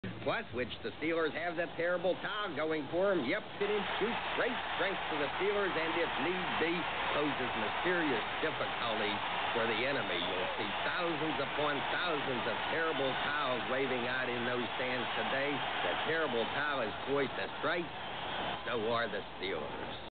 The Terrible Towel is poised to strike, and so are the Steelers
Myron's Birthday is Jan. 23rd... Wave the towel to rally the Steelers to victory in the AFC Championship in Myron's Memory!!!